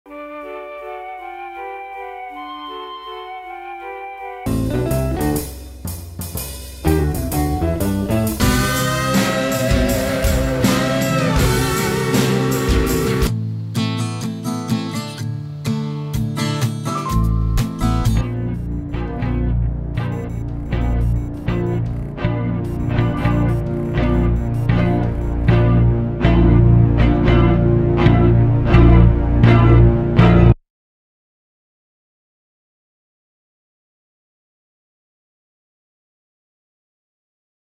A Slightly Psychedelic Countdown : New David Berry Album Coming Soon!
Countdown through 5 tasty samples of all-new songs from my upcoming album.

Putting the finishing touches on album art, video, and final mixes in the next few weeks.